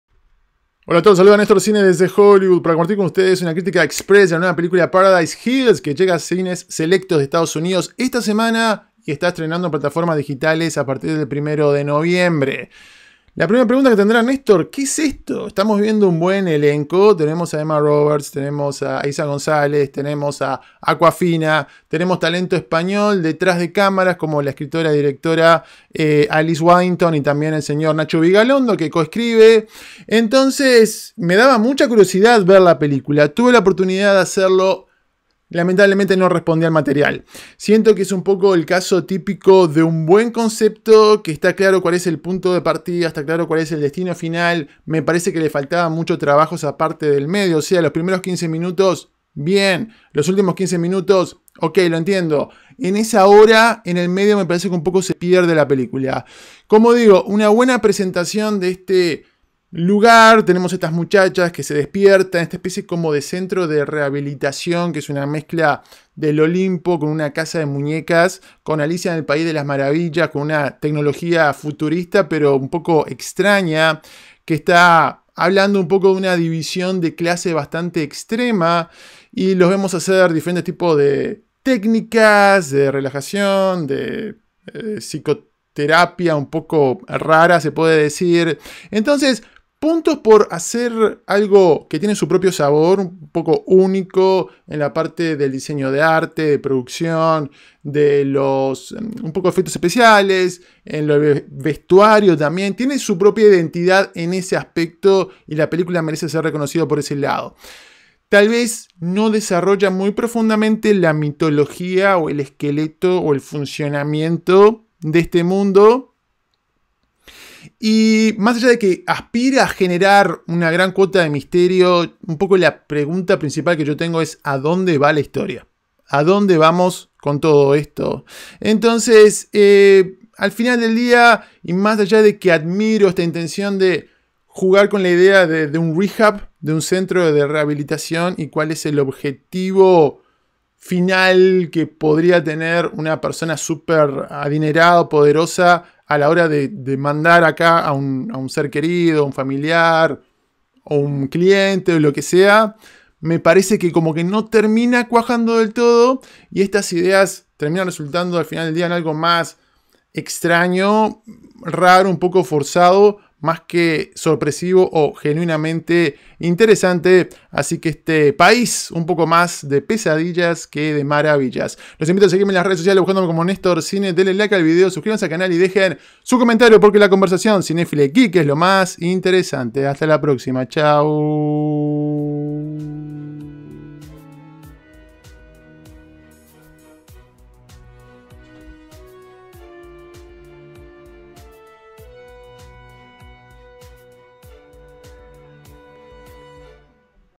Crítica PARADISE HILLS - Reseña de la Película sin Spoilers
Nestor Bentancor (@NestorCine) comparte su opinión crítica de la película PARADISE HILLS (Emma Roberts, Eiza González, Milla Jovovich, Awkwafina) en una reseña sin spoilers.